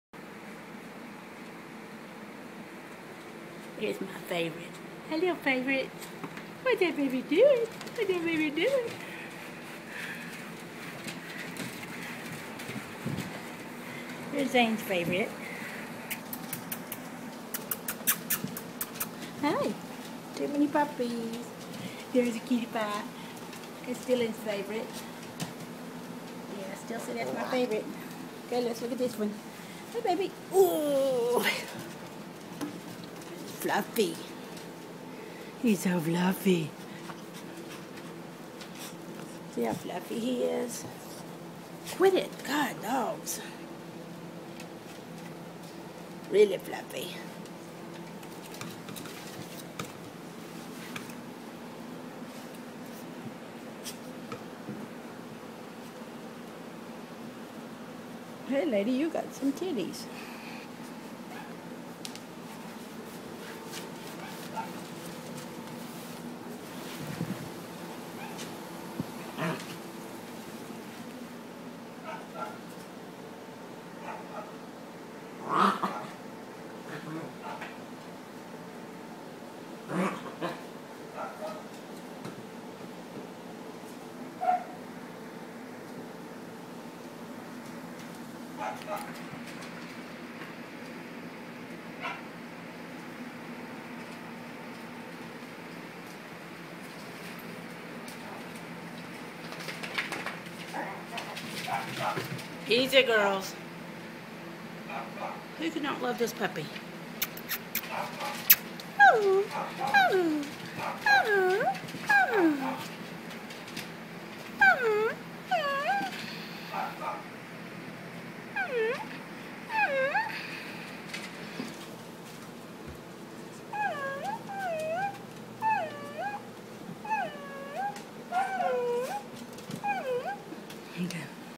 Cutest little Dachshund Mix puppies
Cutest little fluffy dachshund mix puppies EVER.  Little dapple & long haired.
Daddy - Oris - Dachshund Longhair Red White Dapple (Looks like a piebald)
Mama - Fudgy - Chihuahua Dachshund Mix, Black w/white.